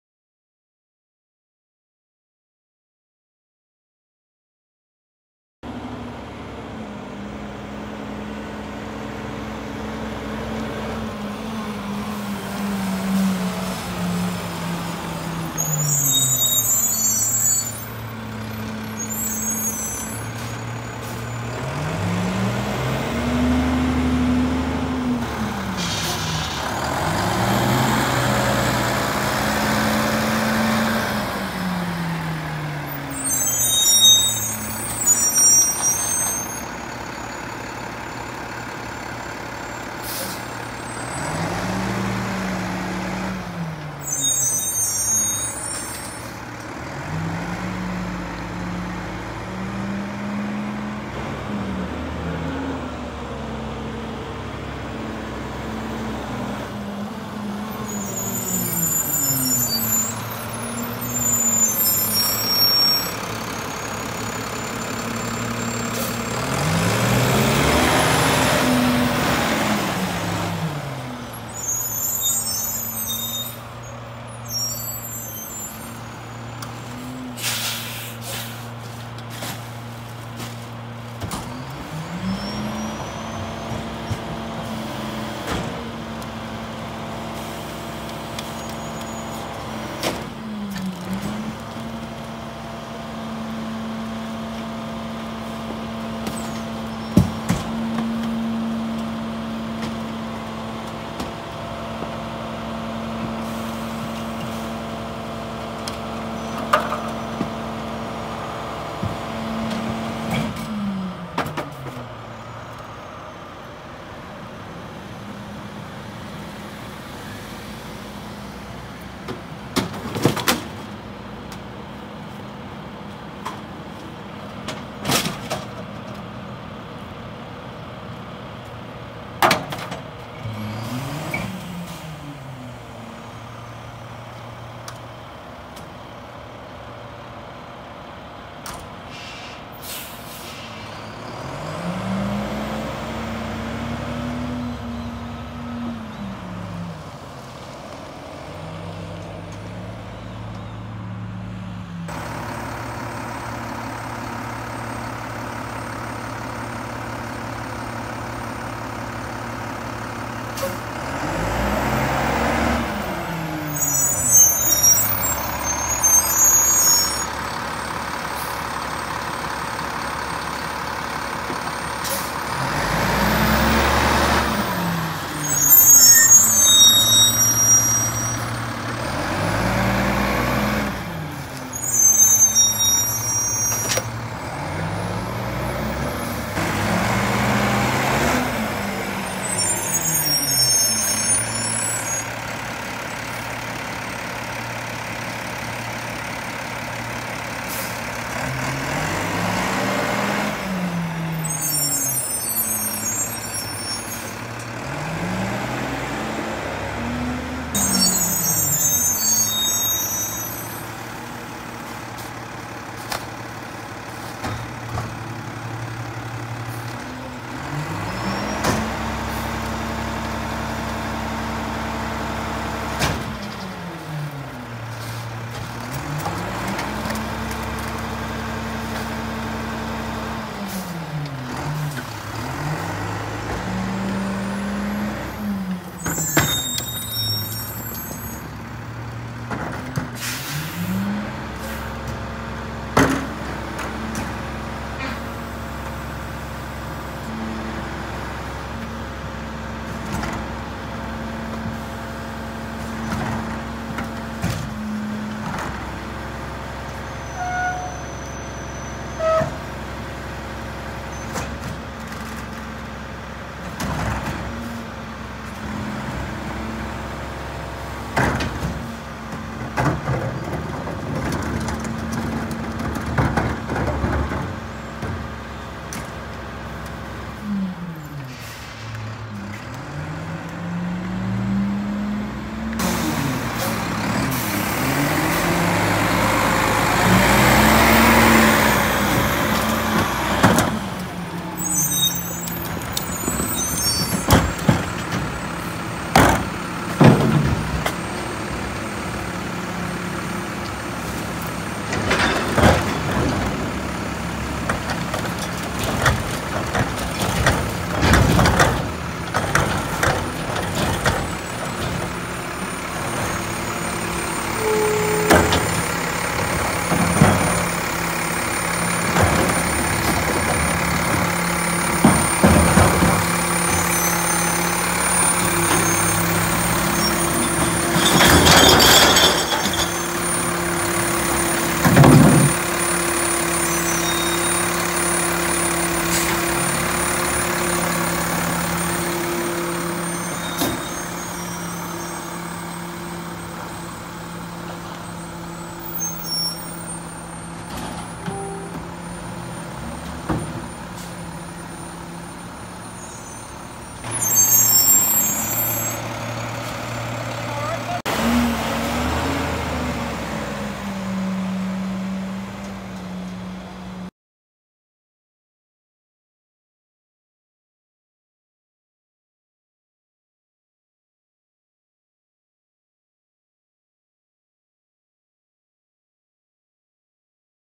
Advanced Disposal • Mack LEU McNeilus Garbage Truck
Here is somewhat of an interesting truck. It was originally from Orange County, Florida, and was shipped up this way back in 2014. This truck has taken one hell of a beating, and is looking super rough as of late. The tipper on it is terrible, and none of the crews that do any of the carted routes like to use it, leaving it pretty much perfect for this pay-as-you-throw bagged route.

This was recorded on one of the last days they collected here, as Waste Connections took over the Carlisle contract as of 1/1/2020. Bags switched to orange and increased from $2 per bag to a few cents under $6, angering a lot of residents that the borough refused a cheaper option, that included keeping Advanced as their hauler, and rolling out carts to everybody. Instead, council voted to hike the price of the bags, and to roll out 35 gal recycling carts to all residences. You can check my videos of Waste Connections collecting both trash and recycle here in Carlisle here:

Connect with IWS15!

Location: Carlisle, Pennsylvania